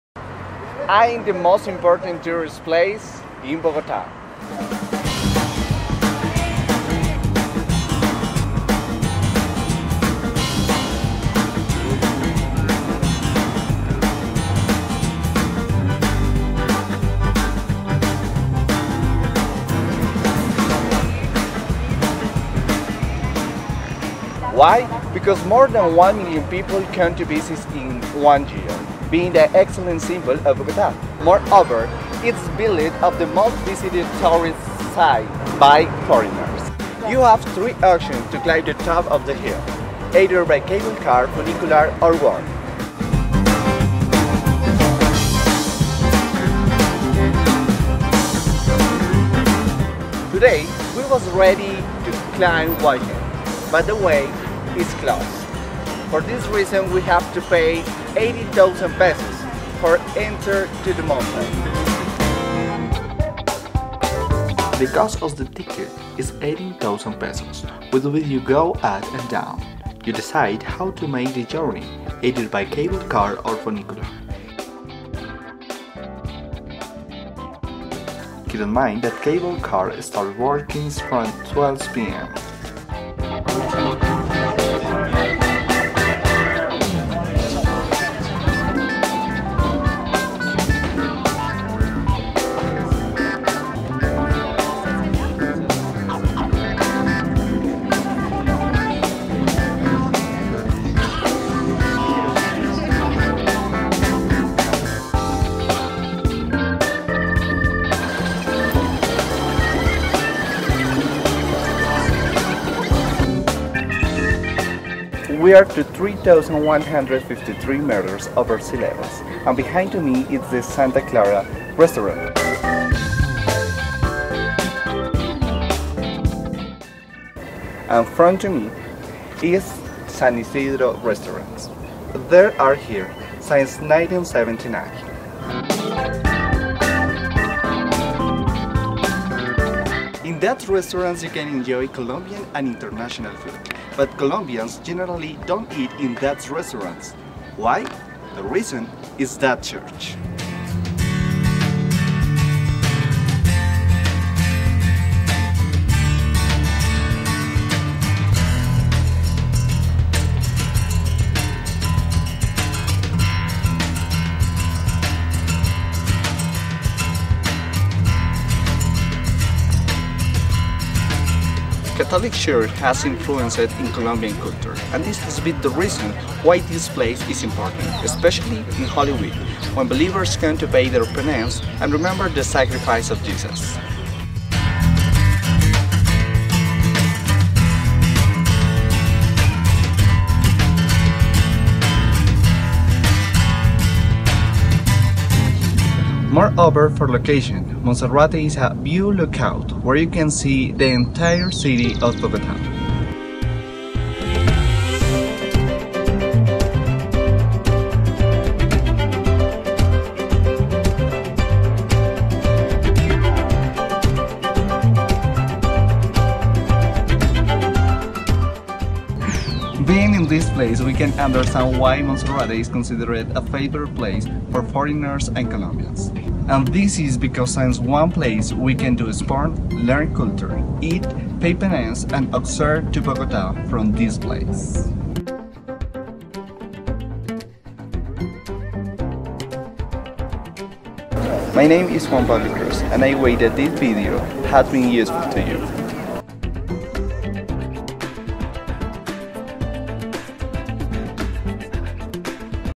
Monserrate
Video made for the World English Online 2016 event at the Externado University. Created by Juan Pablo Cruz Clavijo.